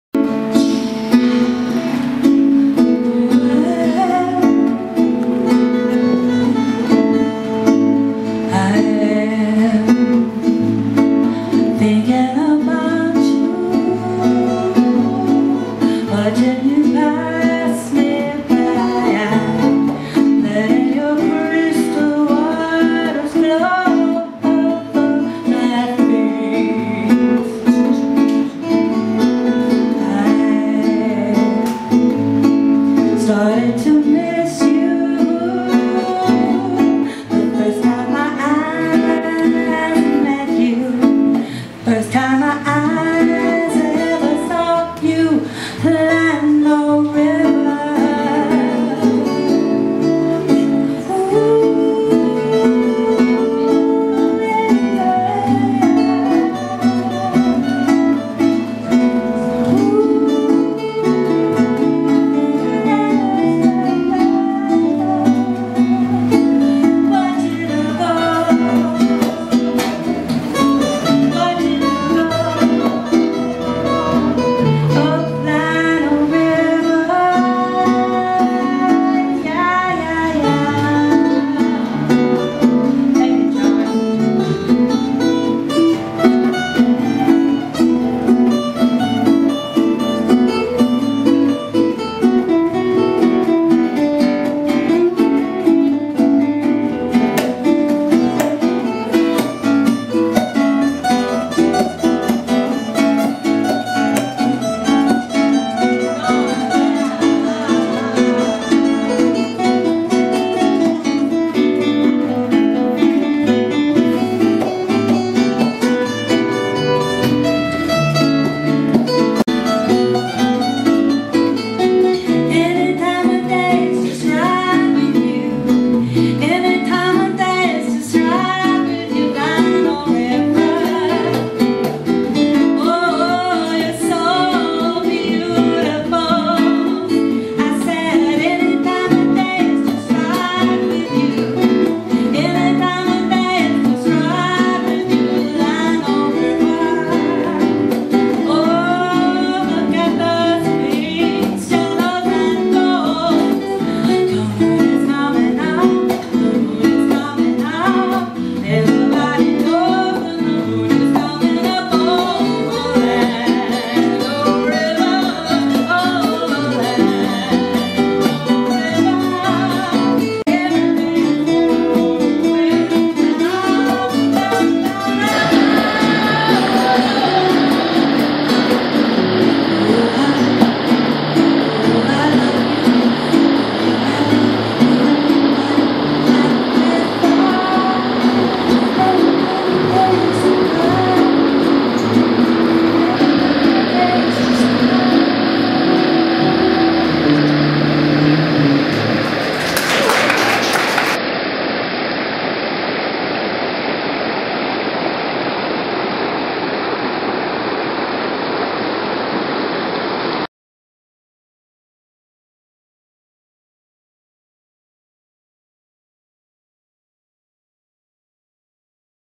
The Llano River
I took these pictures today on a photo adventure at the Llano River.  My sister, Harriet Myrick, wrote an original soundtrack.   Playing with her is John Caballero.  July 22, 2010